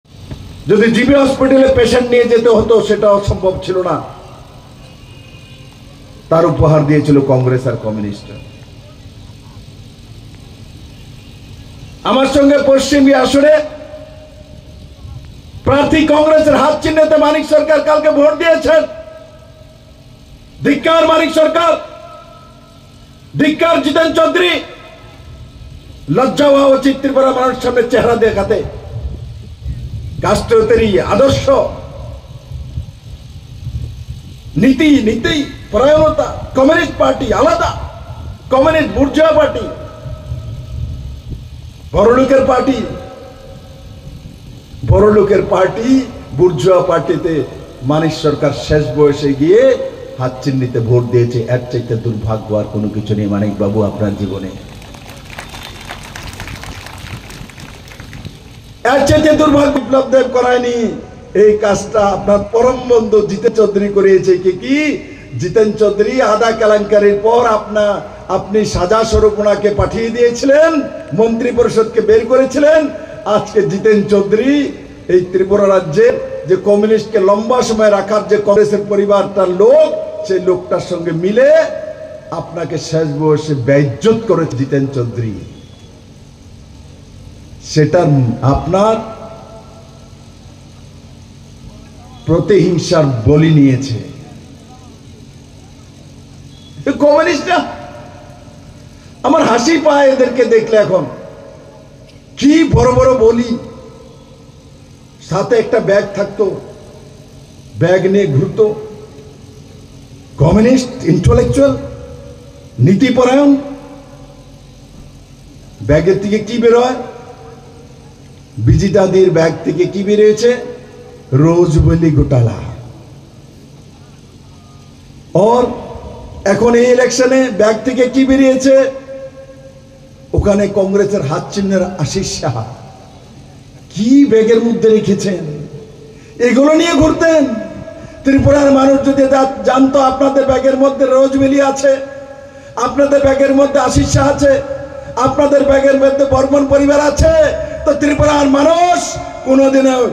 বাগবাসা বিধানসভা কেন্দ্রের জয়নগর উচ্চ মাধ্যমিক বিদ্যালয়ের মাঠে নির্বাচনী জনসভায় বিপ্লব কুমার দেব
শনিবার ধর্মনগর মহকুমার বাগ বাসা বিধানসভা কেন্দ্রের জয়নগর উচ্চ মাধ্যমিক বিদ্যালয়ের মাঠে  নির্বাচনী জনসভায়  বিপ্লব কুমার দেব